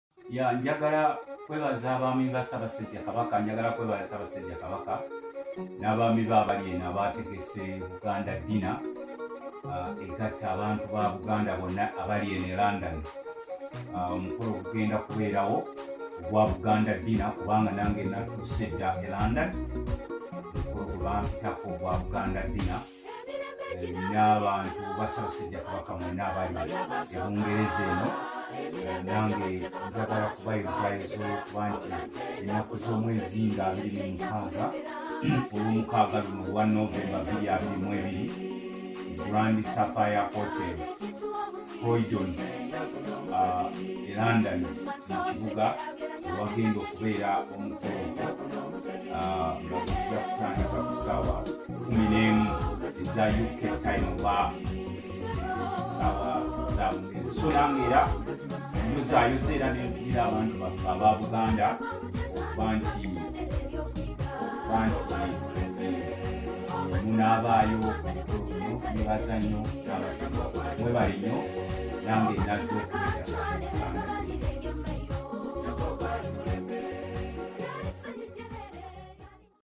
Buganda Dinner 2022 (UK) Promo #5 – Hon Luttamaguzi
The Office of Kabaka's representative in UK & Ireland invites you to the 6th Buganda Dinner (UK) on 26/11/2022 at Grand Sapphire Hotel & Banqueting, 45 Imperial Way, Croydon CR0 4RR.

The Dinner gala brings Buganda & Ugandan Community in the UK and their friends together to network and reminisce on things Buganda, share Buganda history, celebrate Buganda culture and tradition while at the same time discussing ways that will develop our motherland.

Most of the money received from ticket sales is spent on the events costs. Any surplus that will be realised and all the money raised through fundraising on the day will go towards supporting Buganda Kingdom’s projects. The surplus and funds raised from Buganda Dinner (UK) 2021 were used to support Buganda Kingdom’s Borehole and the County/sub county bicycle projects.